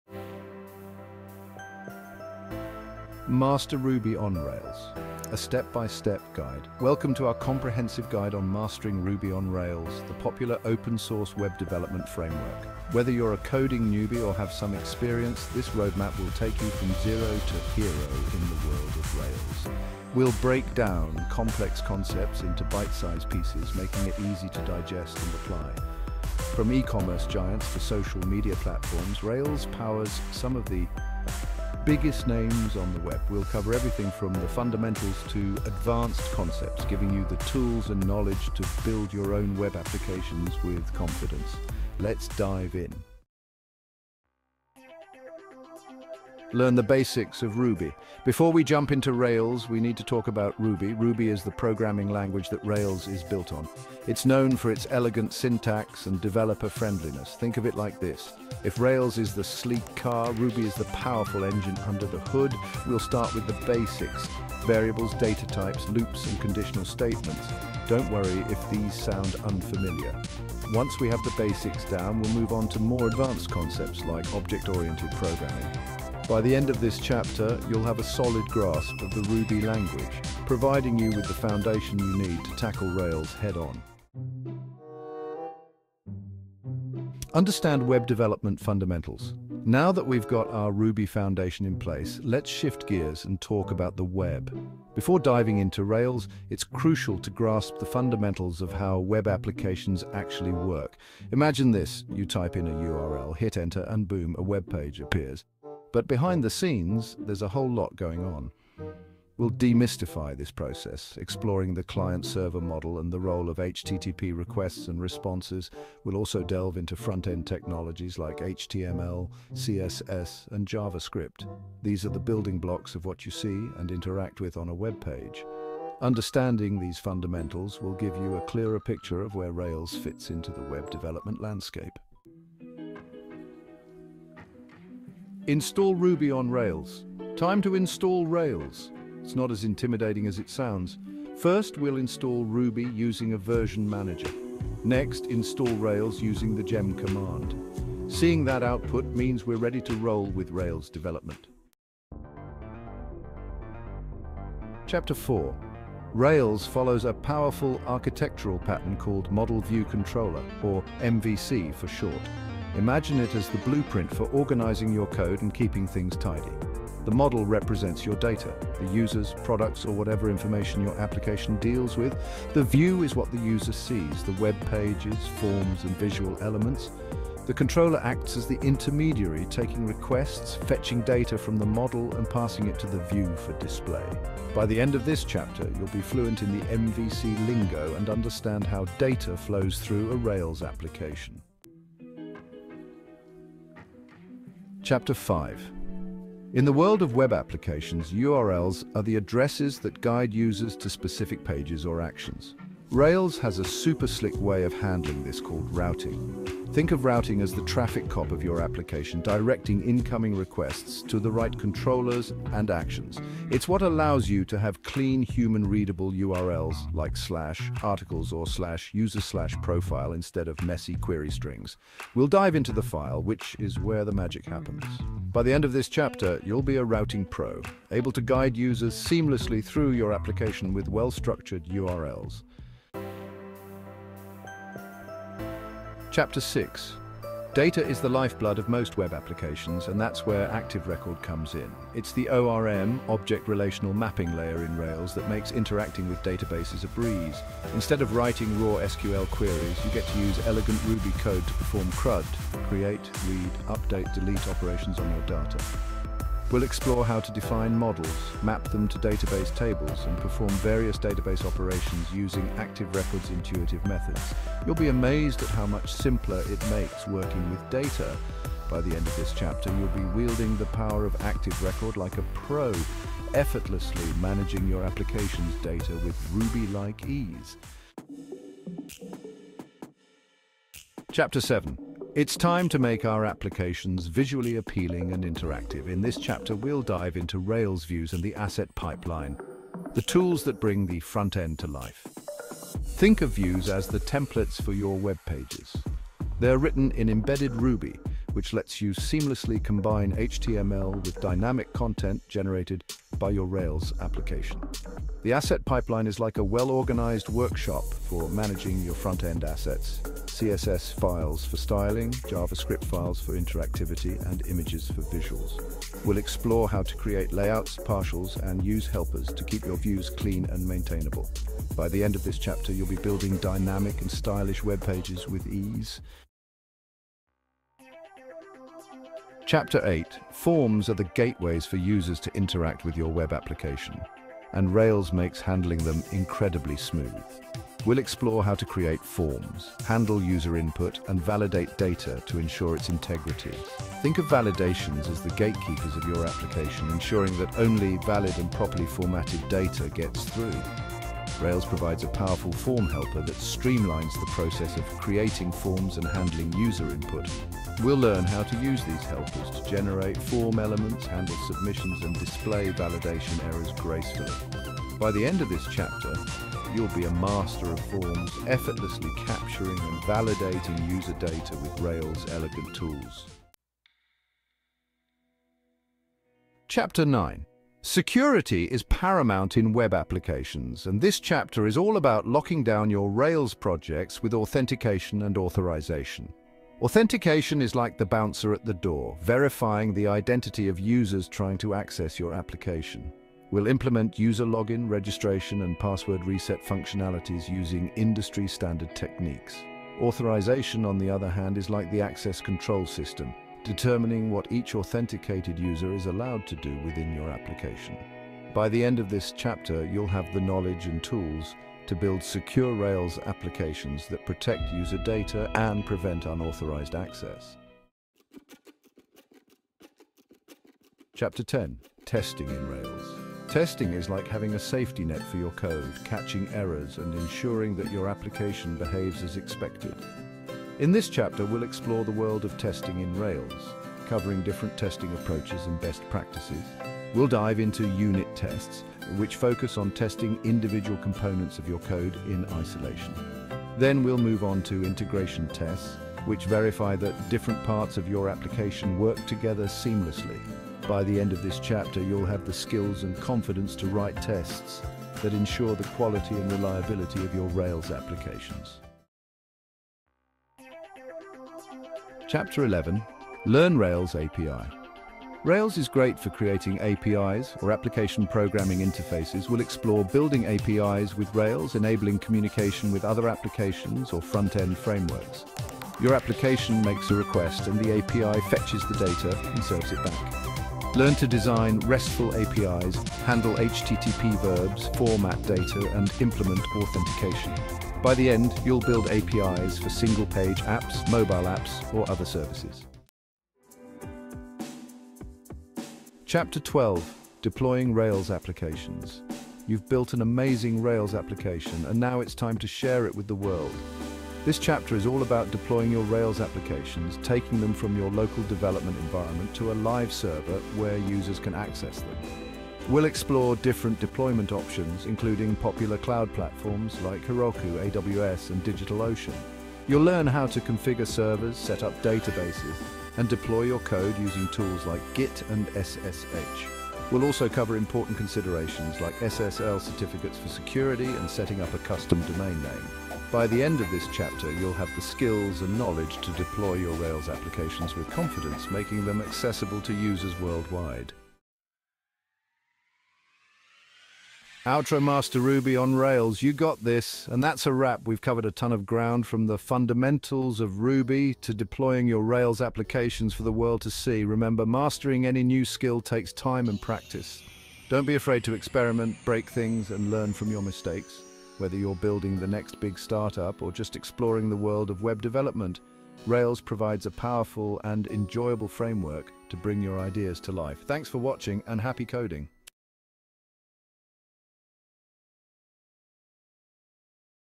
A Complete Roadmap To Master Ruby on Rails 2024 Backend Development
Timecodes:-
0:00 - A Step-by-Step Guide
0:43 - Learn the Basics of Ruby
1:24 - Understand Web Development Fundamentals
2:12 - Install Ruby on Rails
2:31 - Learn MVC Architecture
3:11 - Learn Rails Routing
3:52 - Master Active Record (ORM)
4:37 - Understand Rails Views & Asset Pipeline
5:26 - Master Rails Forms & Validations
6:13 - Master Authentication and Authorization
6:57 - Testing in Rails
7:39 - Learn Rails API
8:13 - Deploying Rails Applications
9:01 - You Got This!